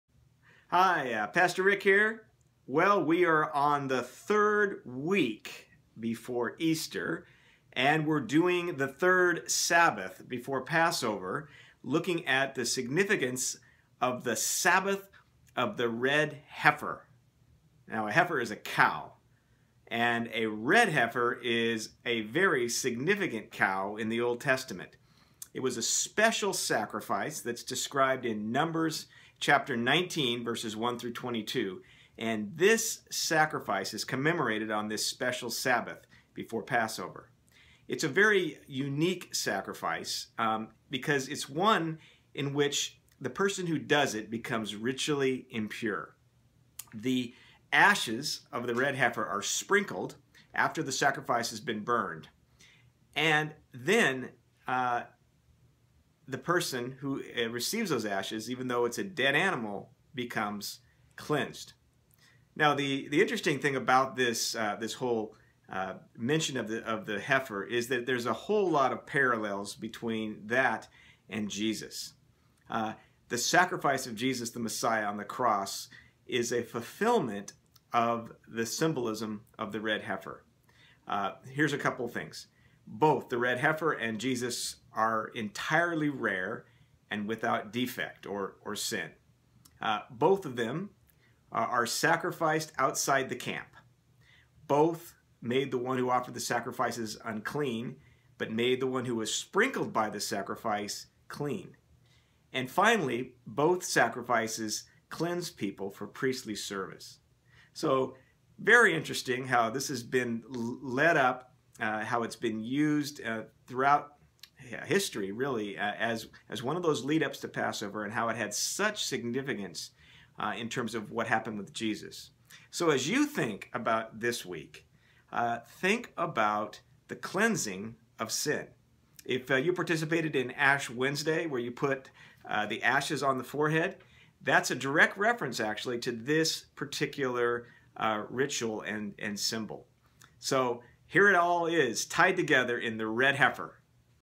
Preparing for Easter - Week 4
Rev. Rick Dietzman teaches about Lent